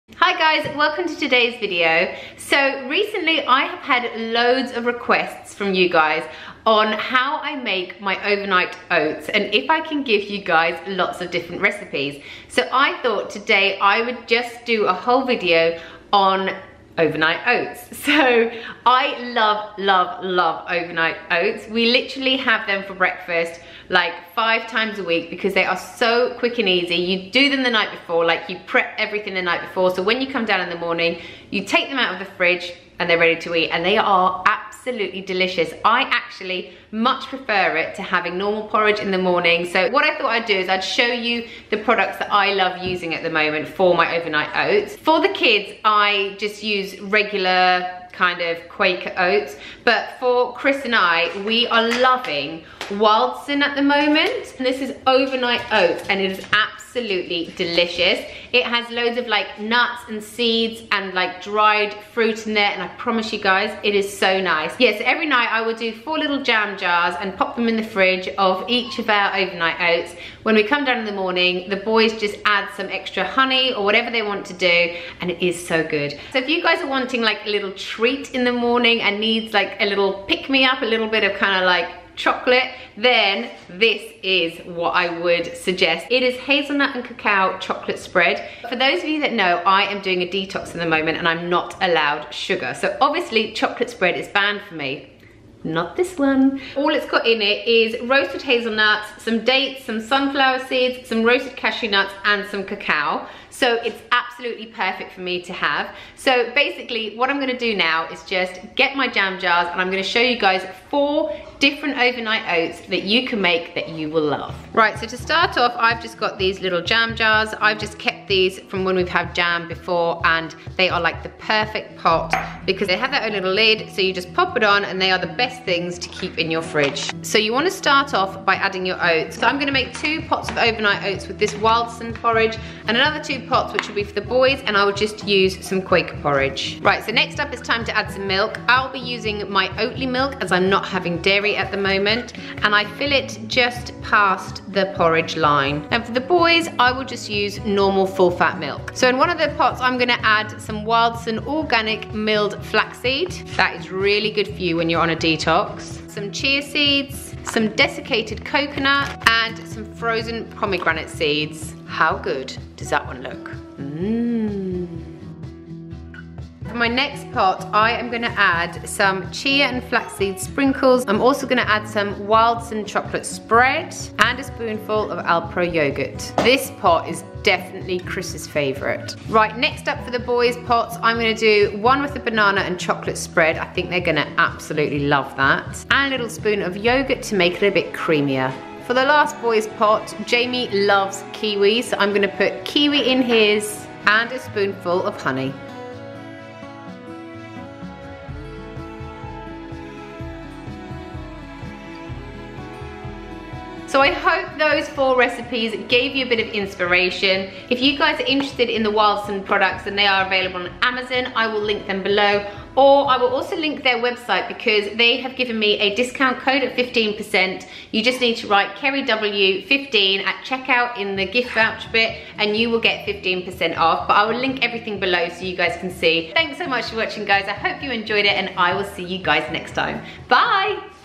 OVERNIGHT OATS 4 DELICIOUS WAYS | HEALTHY BREAKFAST IDEAS | KERRY WHELPDALE
Today I am showing you guys how to make 4 super easy overnight oat recipes that your whole family will love!

Chocolate spread on Amazon